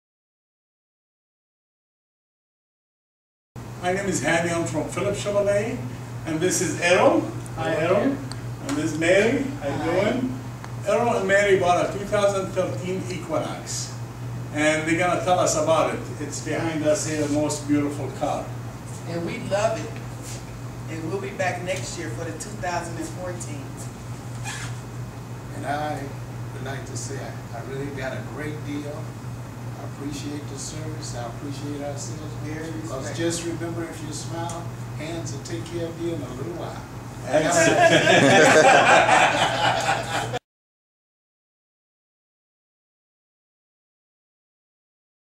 2013 Chevy Equinox Review - Phillips Chevy
Earl & Mary Miles  from Dolton,IL purchased a 2013 Chevy Equinox from Phillips Chevrolet located at 9700 West Lincoln Highway in Frankfort, IL. Sales consultant Hani Sahoury . Visit us at phillipschevy.com.